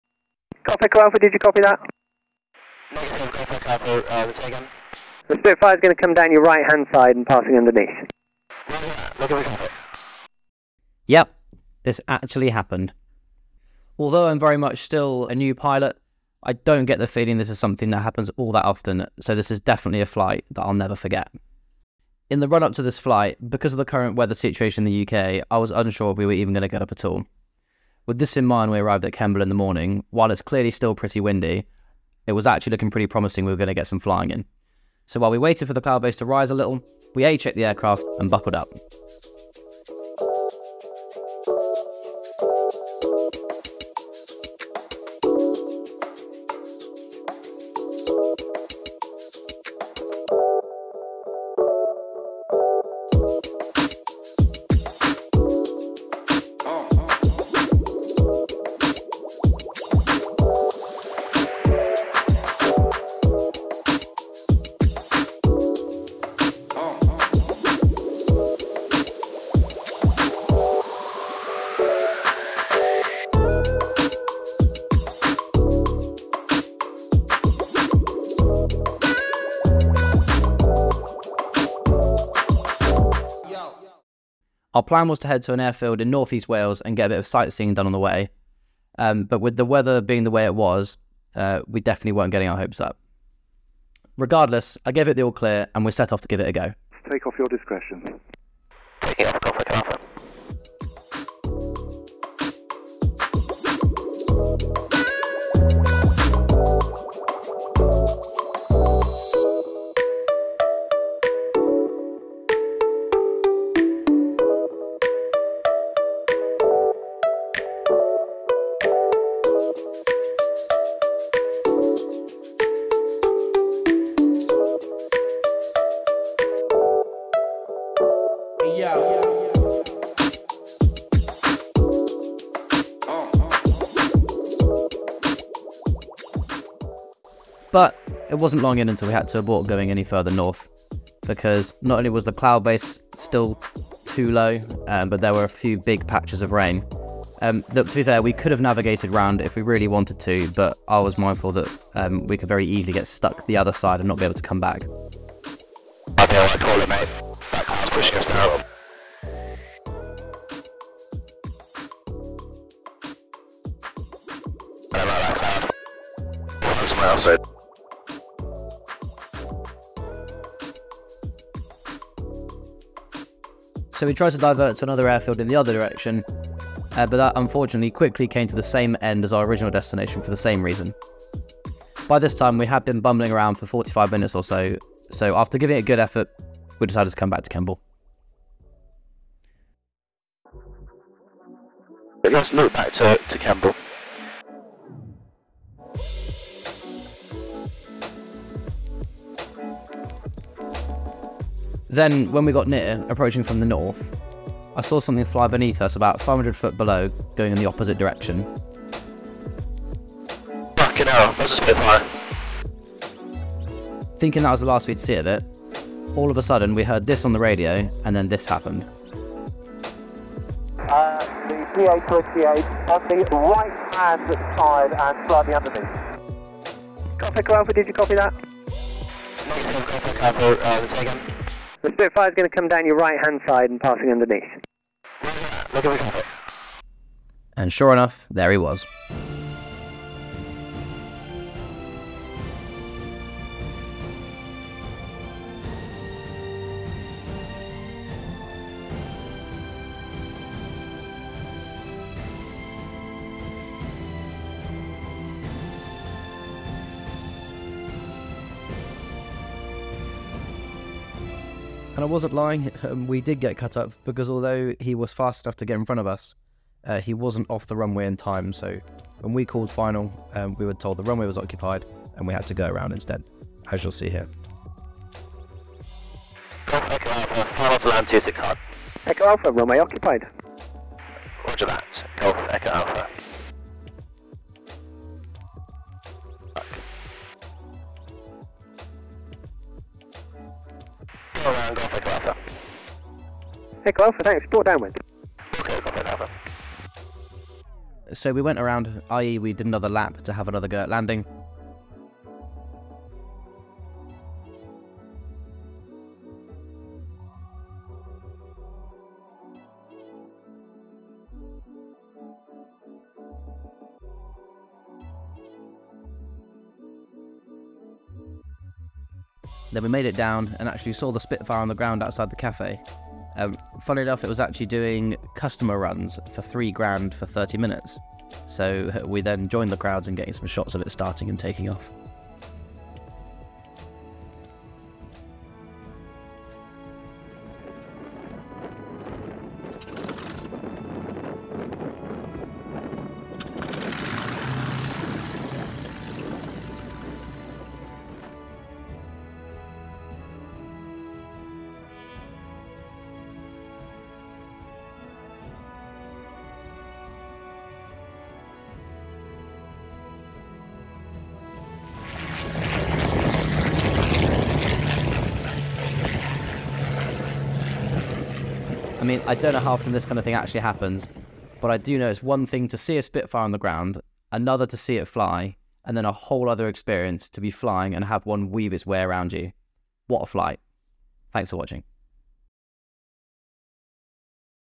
New Pilot CUT UP... by a SPITFIRE
Here's us coming into land back at Kemble airfield after another weather curtailed flight and then seeing this. Still can't really believe it.. especially being only my 3rd flight after getting my licence. Definitely one to remember! Hope you enjoy!

Thanks to Hugh for helping out filming, give him a follow: @F5HPP